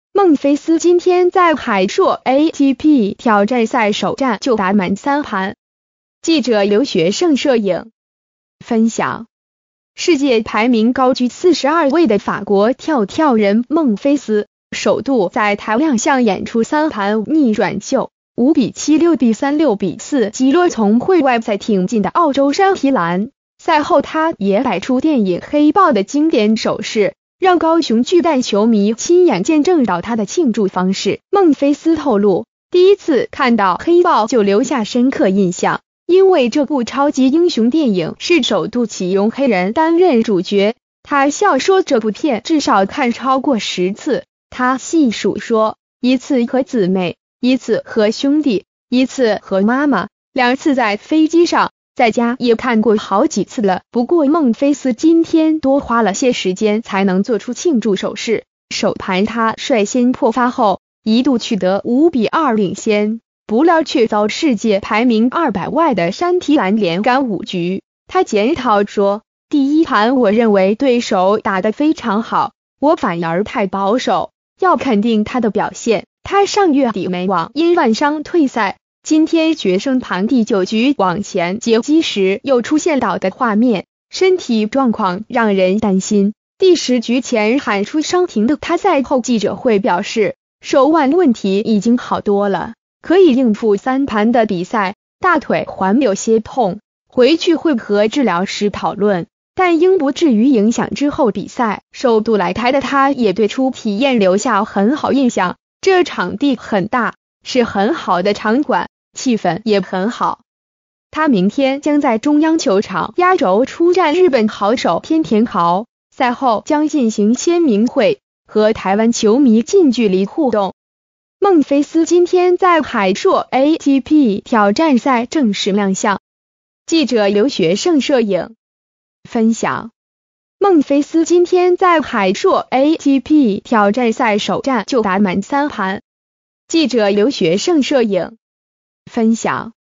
海碩男網／孟非斯超愛「黑豹」 電影重複看不停
孟非斯今天在海碩ATP挑戰賽首戰就打滿3盤。記者劉學聖／攝影 分享        世界排名高居42位的法國「跳跳人」孟非斯，首度在台亮相演出3盤逆轉秀，5：7、6：3、6：4擊落從會外賽挺進的澳洲山提蘭，賽後他也擺出電影《黑豹》的經典手勢，讓高雄巨蛋球迷親眼見證到他的慶祝方式。

發表評論評論如下。